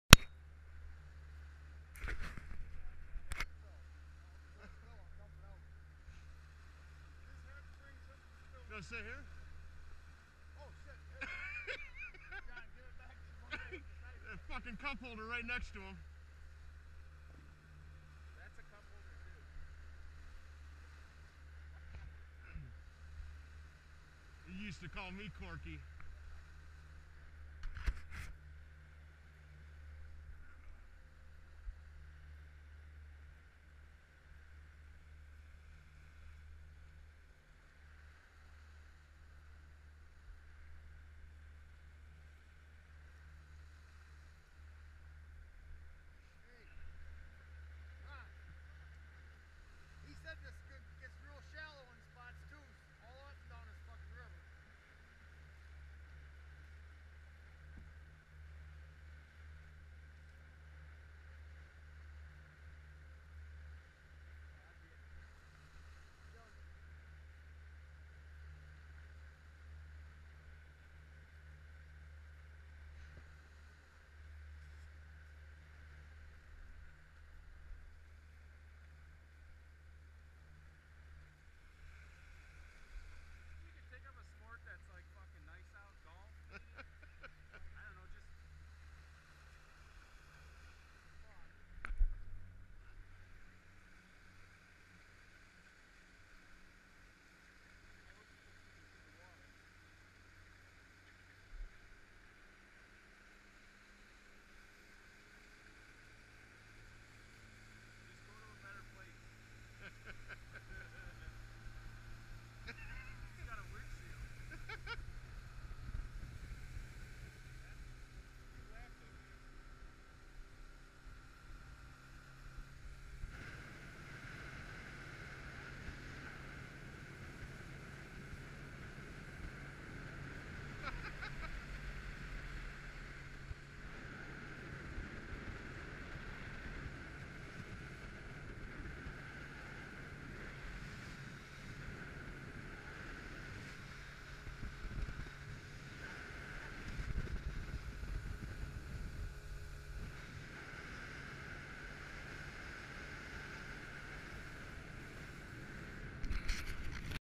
Saginaw River fishing
Maiden voyage for Dads new Lund in the Saginaw River. March 6th, 2016.